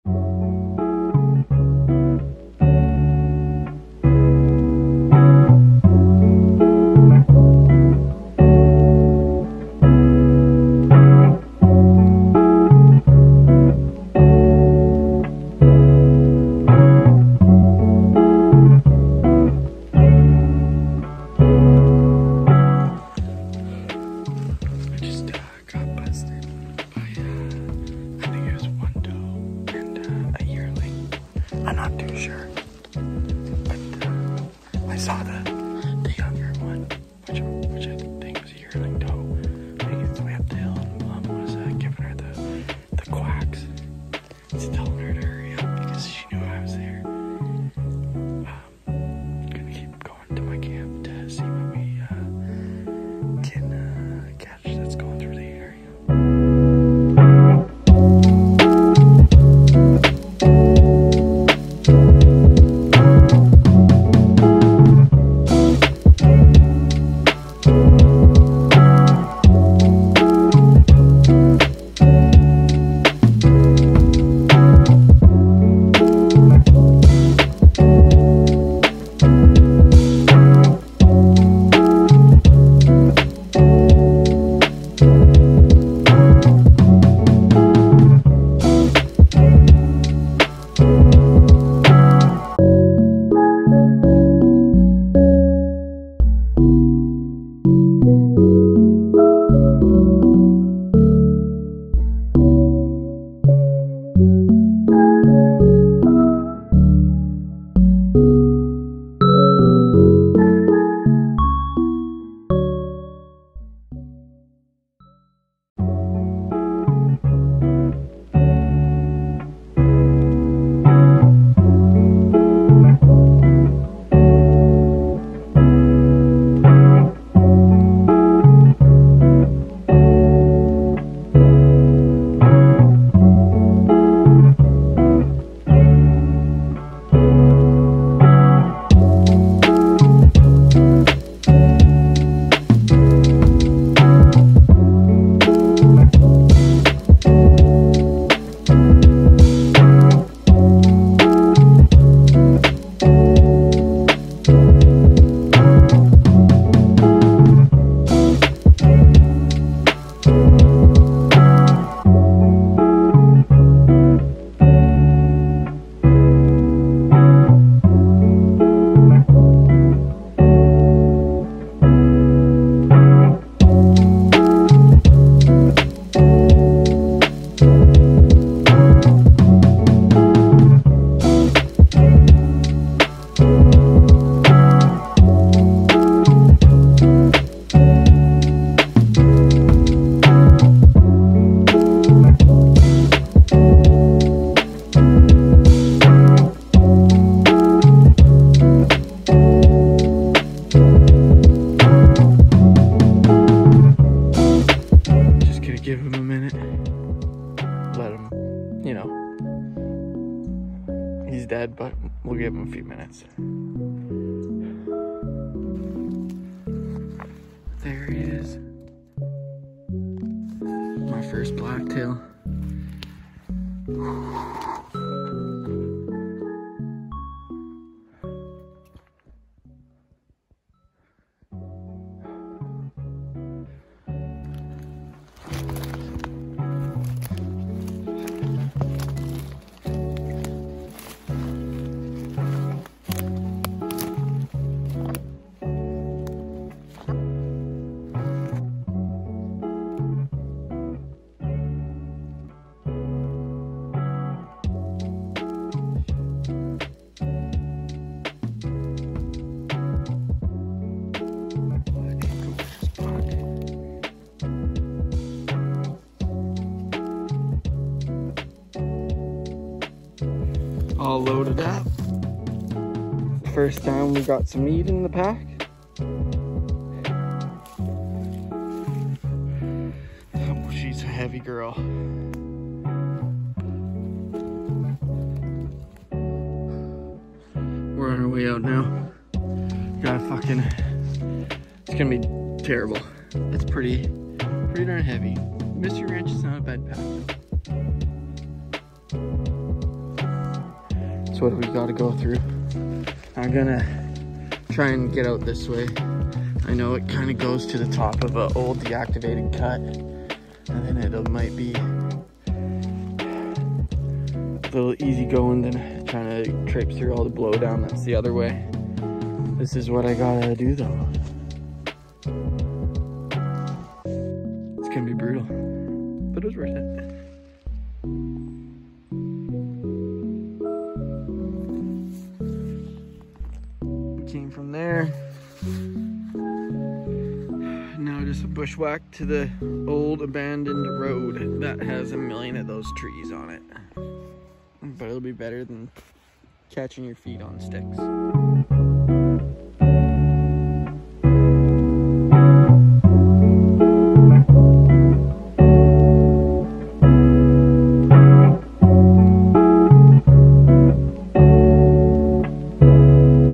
2021 blacktail deer hunting
Hunting Columbia blacktail in region 2